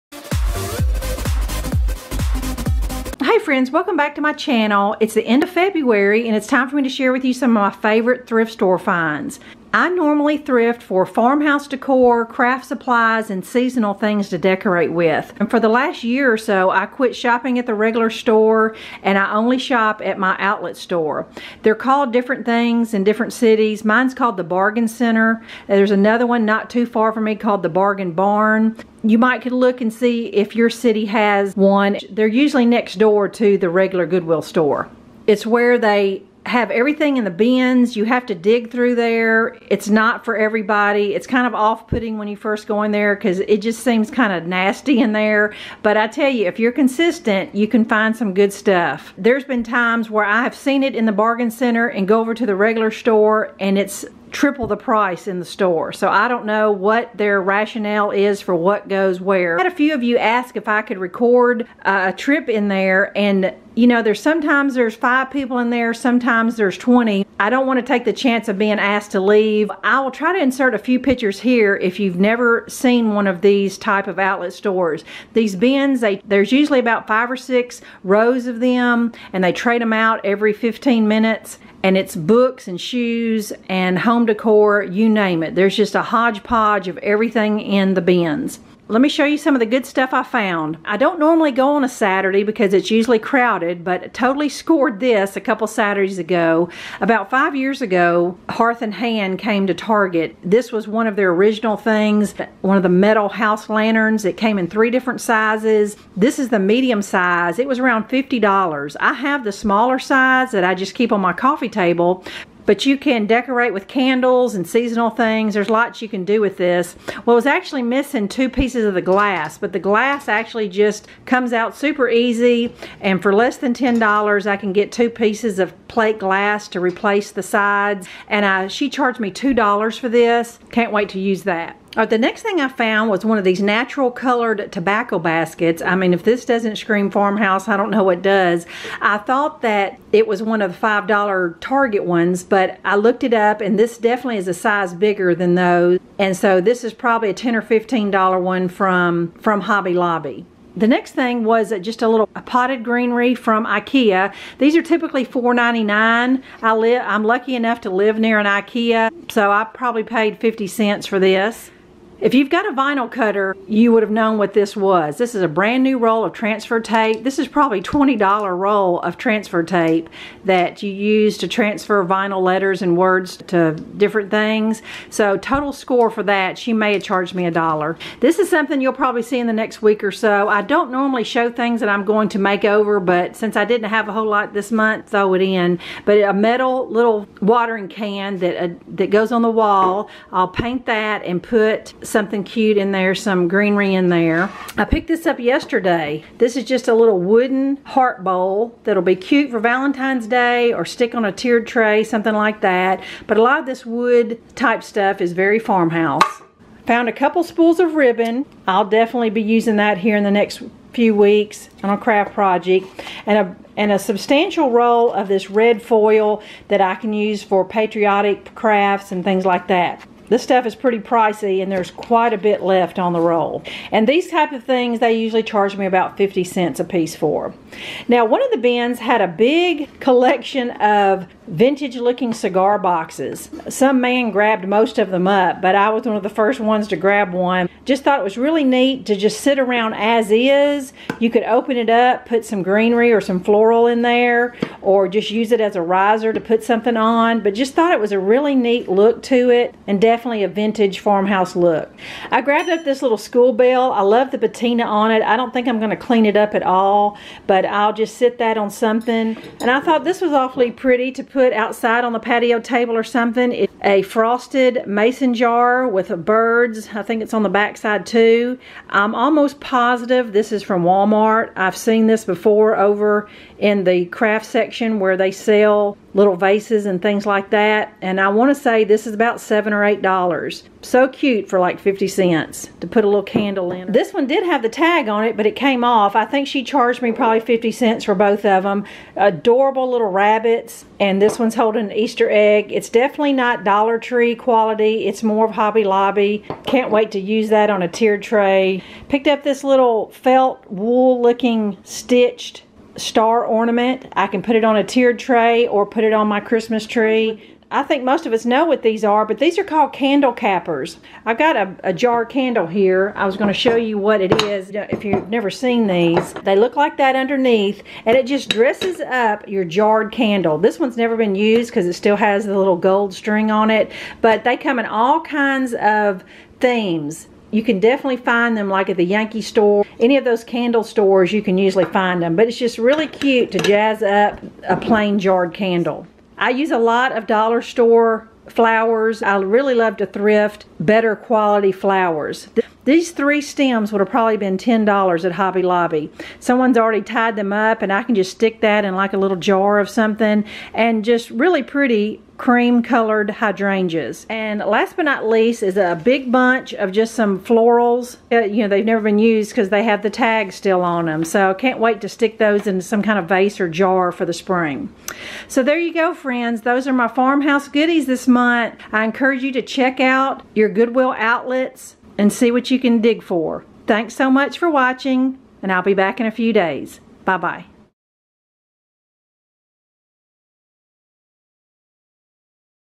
Goodwill Bargain Center Haul February 2020
I found some great farmhouse finds this month in the Goodwill bins.